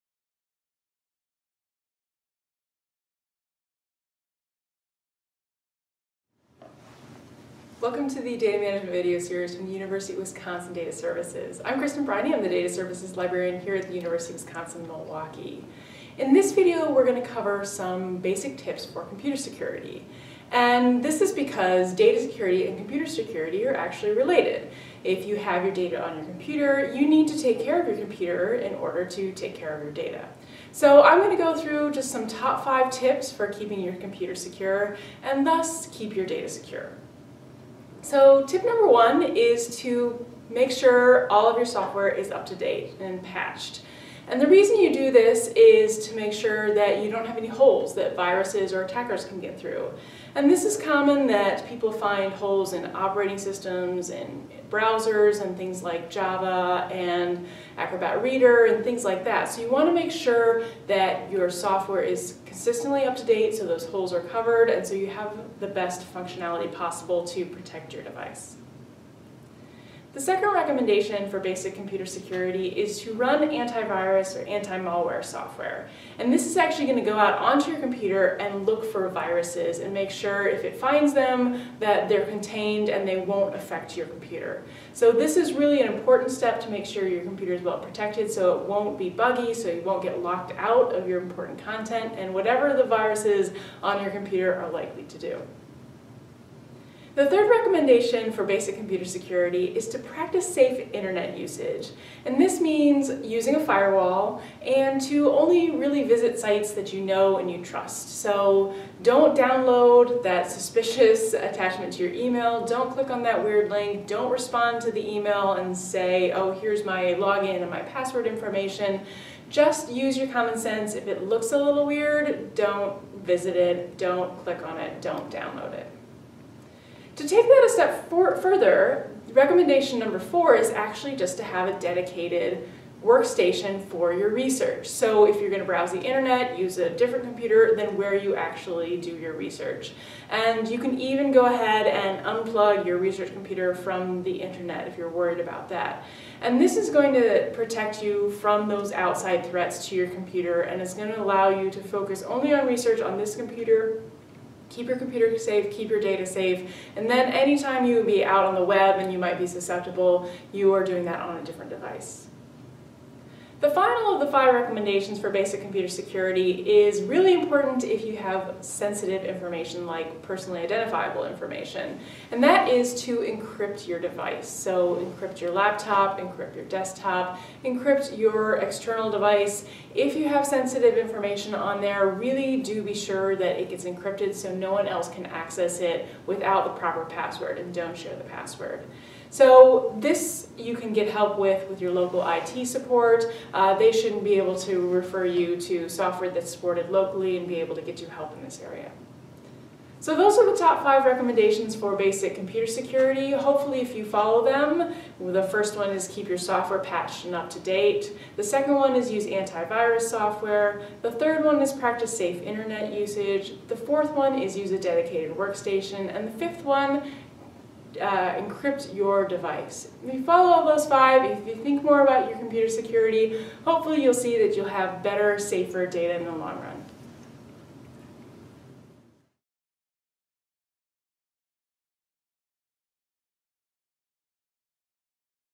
Data Management: Basic Computer Security
Part of keeping your research data safe is keeping your computer safe. This video covers five basic tips for keeping your computer more secure.